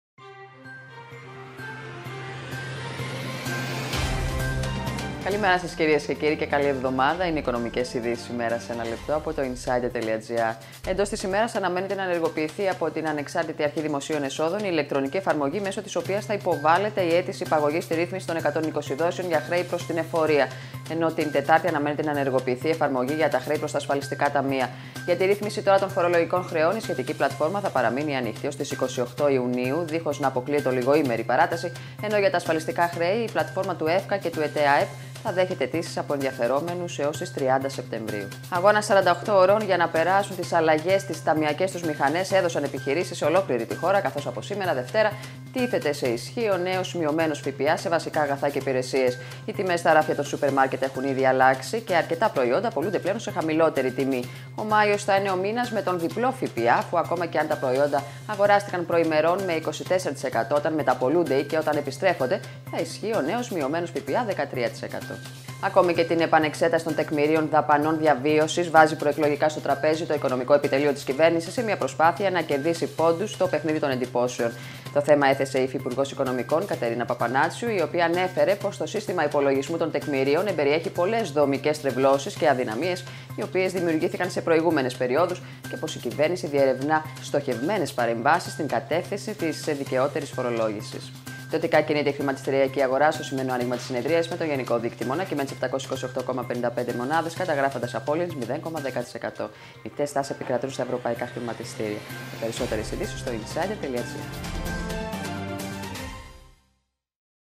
Η οικονομία σε 1' από το insider.gr - 20/5/2019
-120 δόσεις: Σήμερα προς εφορία - Τετάρτη προς ταμεία

-Αλλαγές στα τεκμήρια δαπανών διαβίωσης προαναγγέλλει προεκλογικά η κυβέρνηση

-Μείωση του ΦΠΑ: Σε ποια προϊόντα αλλάζουν οι τιμές από σήμερα